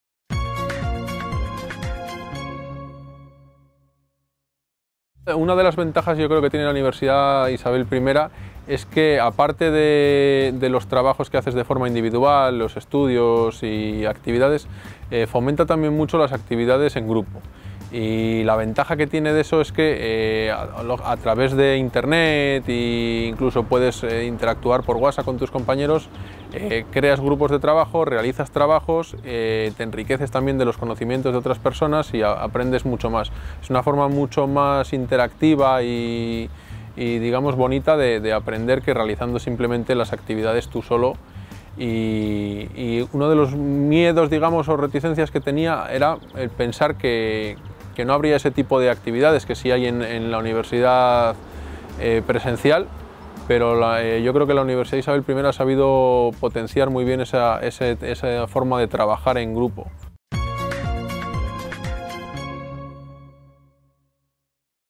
Ventajas de estudiar ADE en la Universidad Isabel I
Infórmate sobre nuestro Grado en Administración y Dirección de Empresas (ADE)

El estudiante del Grado de ADE (Administración y Dirección de Empresas), David Rebollo, nos habla de la importancia del trabajo en equipo durante el aprendizaje y de cómo la metodología de la Universidad Isabel I permite hacerlo con facilidad.

¿Te apasiona la gestión? ► Infórmate de nuestro Grado online en ADE